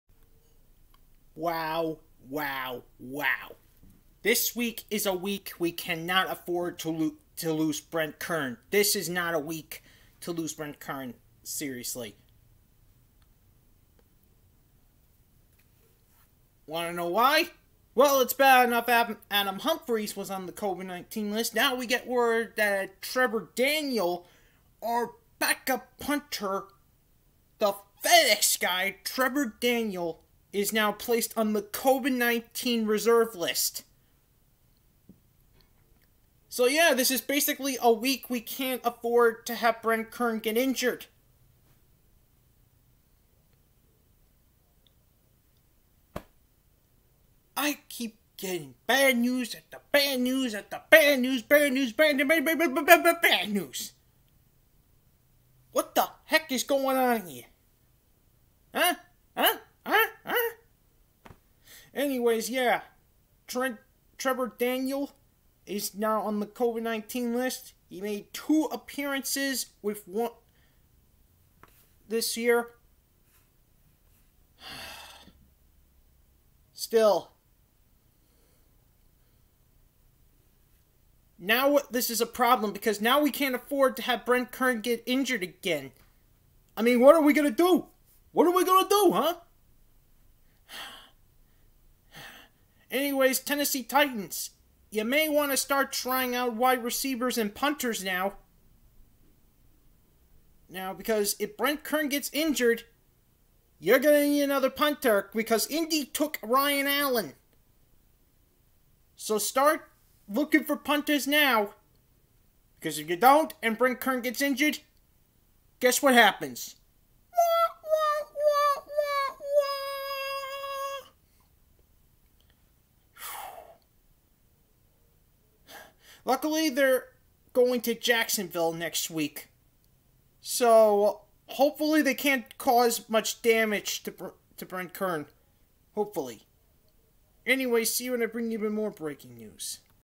TENNESSEE TITANS CAN'T AFFORD TO LOSE BRETT KERN NOW!!
Breaking News: Tennessee Titans PS P Trevor Daniel has been placed on the COVID-19 reserves list!